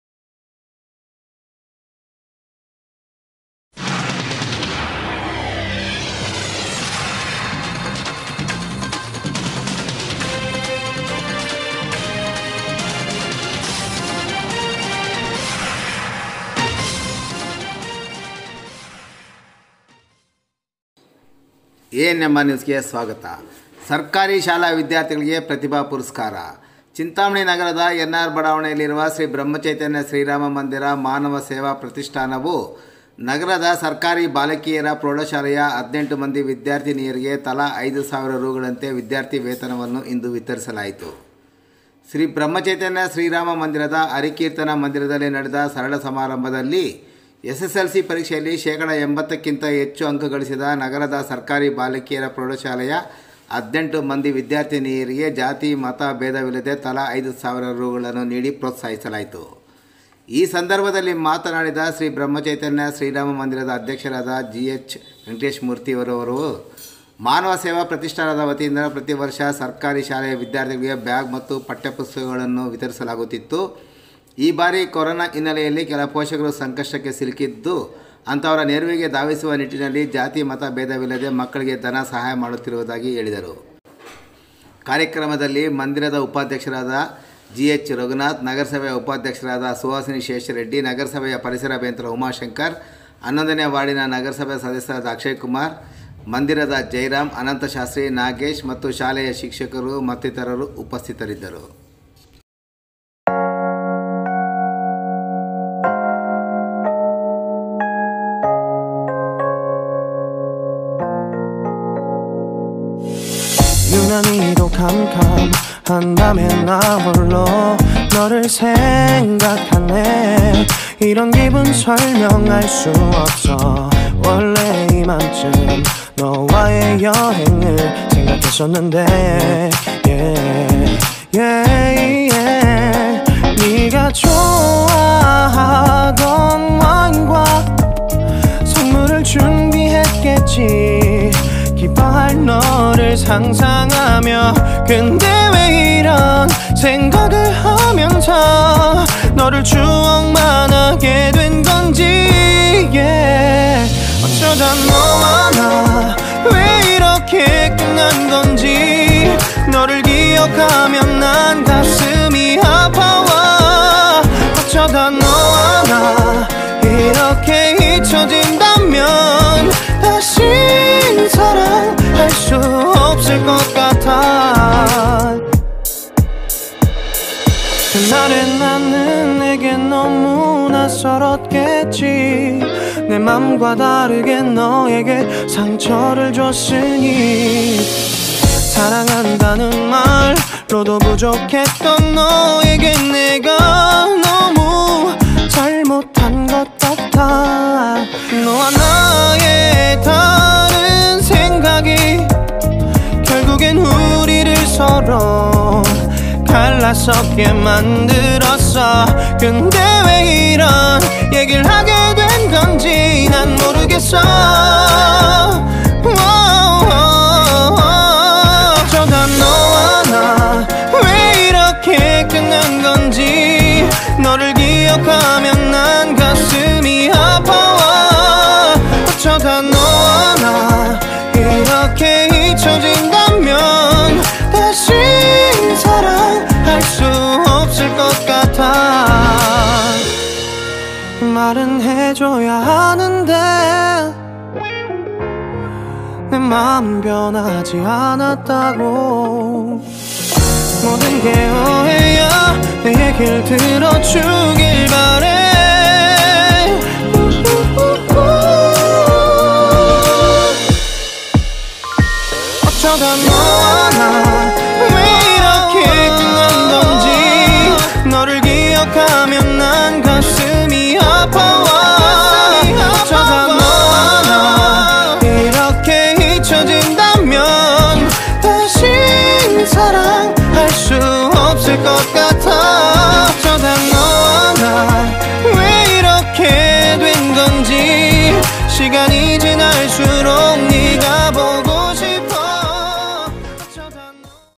ಚಿಂತಾಮಣಿ ನಗರದ ಶ್ರೀರಾಮ ಮಂದಿರದ ಮಾನವ ಸೇವಾ ಪ್ರತಿಷ್ಠಾನದಿಂದ ಸರಕಾರಿ ಶಾಲೆಗಳಿಗೆ ಪ್ರತಿಭಾ ಪುರಸ್ಕಾರ
ANMR News chintamani# ಚಿಂತಾಮಣಿ ನಗರದ ಶ್ರೀರಾಮ ಮಂದಿರದ ಮಾನವ ಸೇವಾ
 ಪ್ರತಿಷ್ಠಾನದಿಂದ  ಸರಕಾರಿ ಶಾಲೆಗಳಿಗೆ ಪ್ರತಿಭಾ ಪುರಸ್ಕಾರ